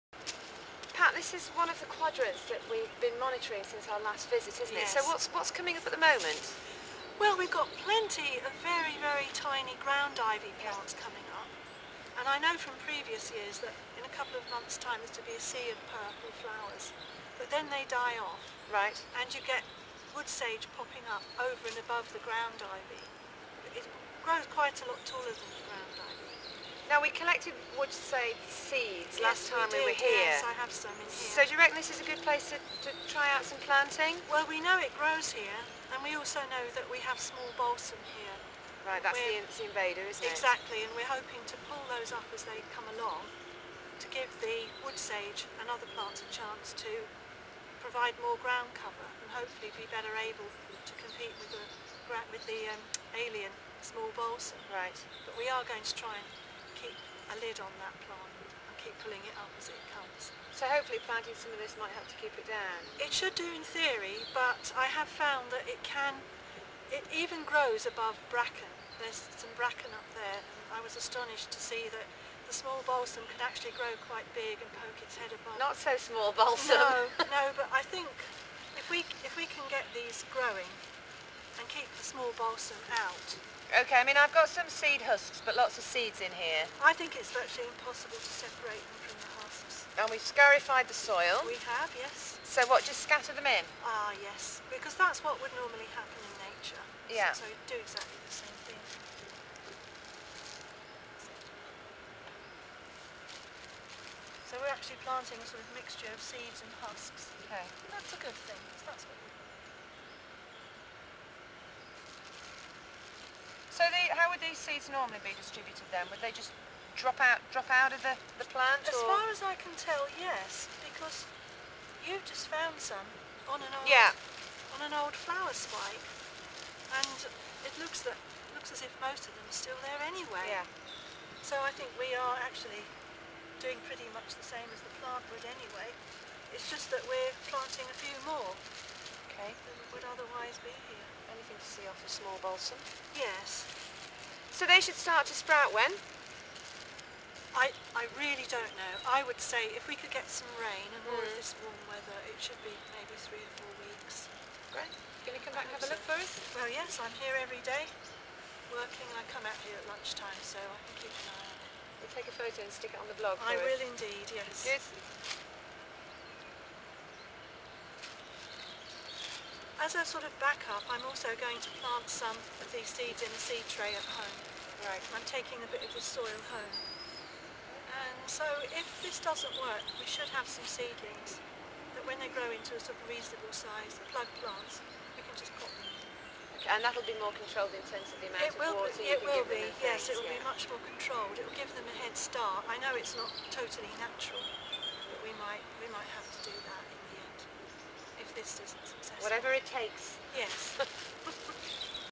Quadrat survey
Planting wood sage and quadrat survey